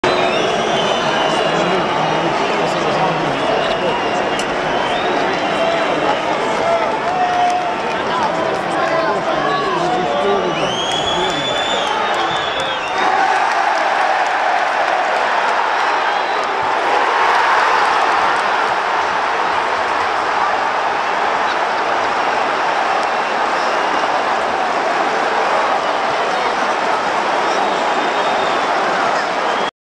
penalty foul from Eboue on Lucas after liverpool's free-kick
free kick from liverpool followed by eboue's foul on lucas